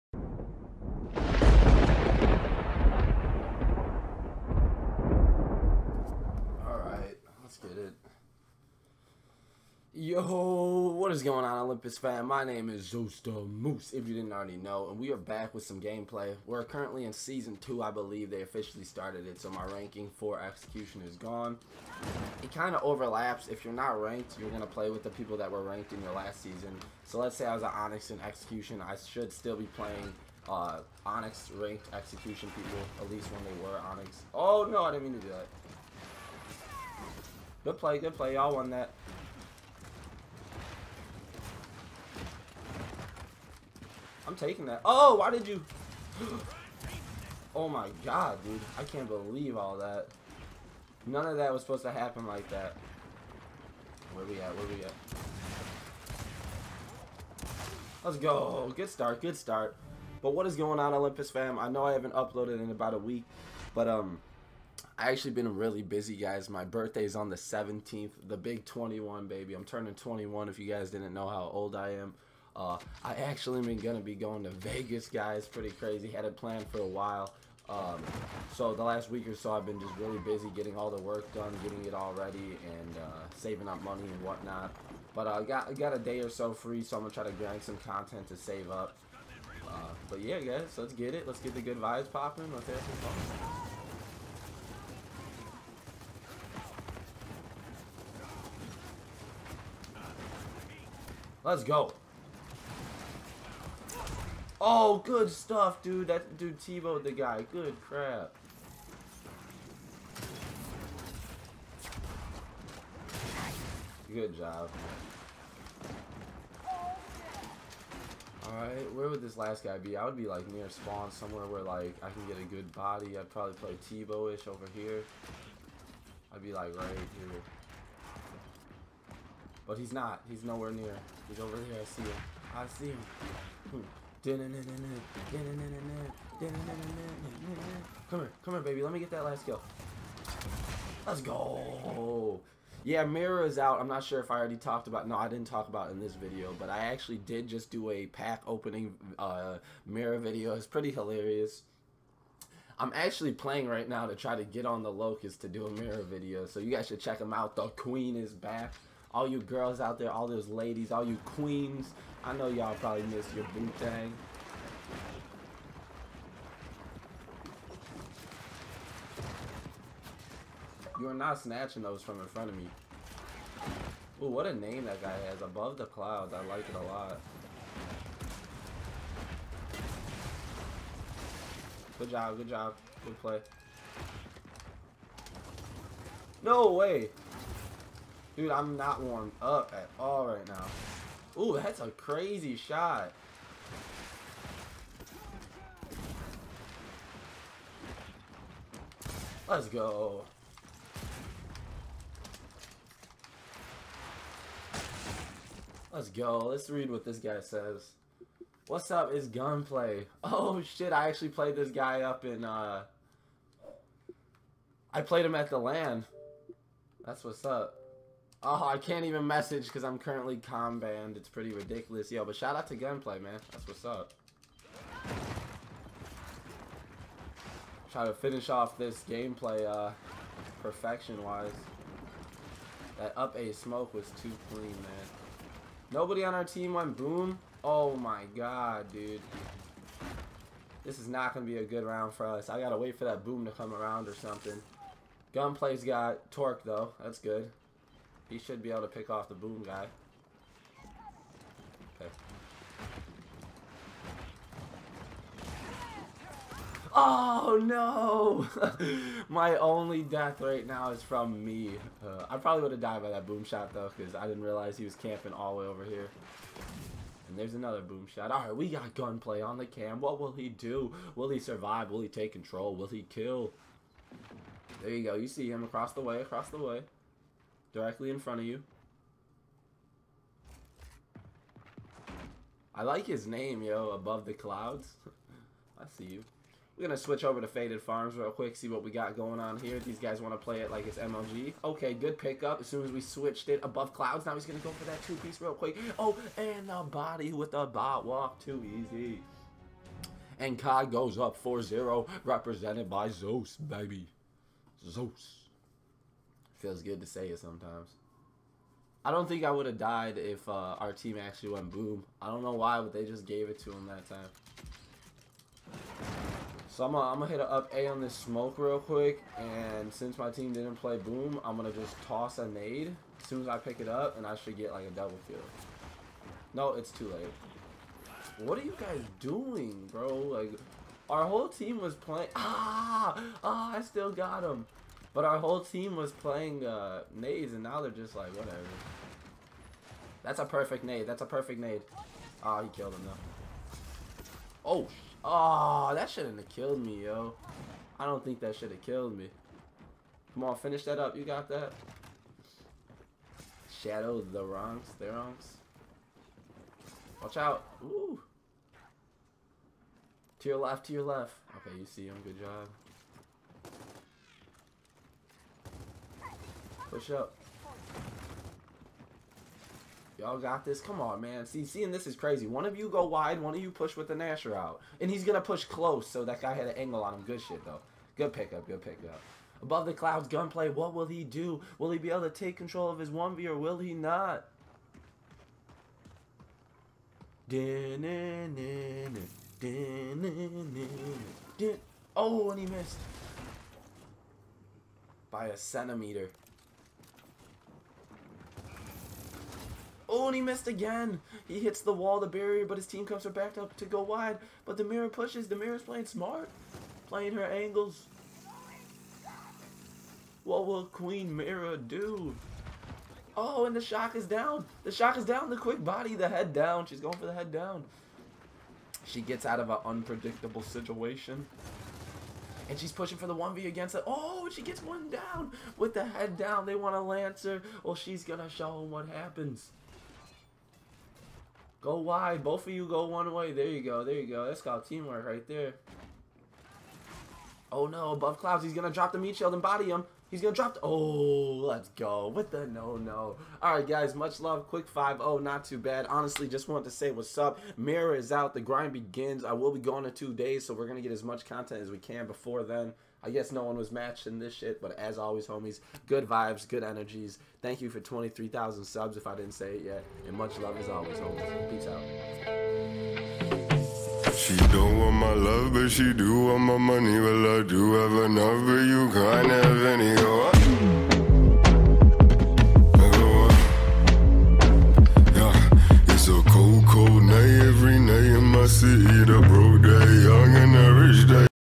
Ranks Reset! Season 2 Begins - Road to Execution Diamond?!
YOOO! had to follow up that pack video with some actual gameplay because number 1, ya'll know how I dislike only pack based videos lmao but I had to share this with you guys! and 2, all YALL WANT IS THAT FIYUH GAMEPLAY! lmao enjoy homies.